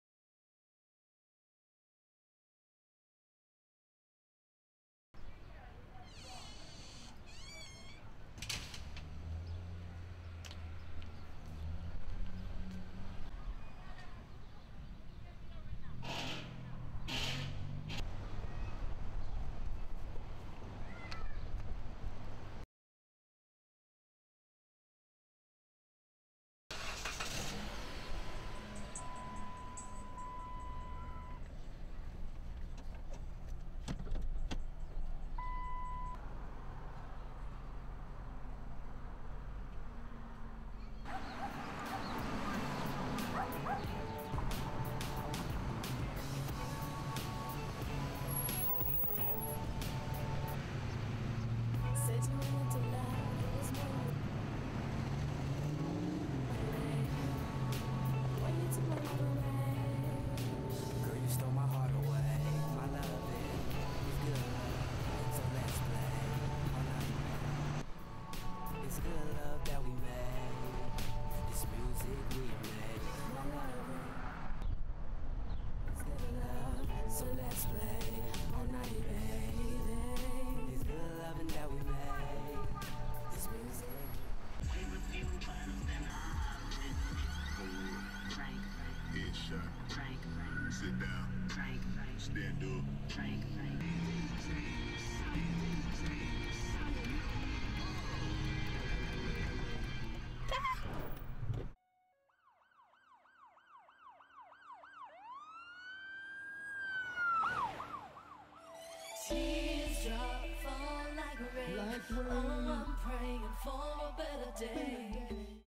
Reality Check2 Don't text and Drive
Get the Message!!Don't text on the Road..Safety first..Follow the Rules of the Road..Film&Video Production.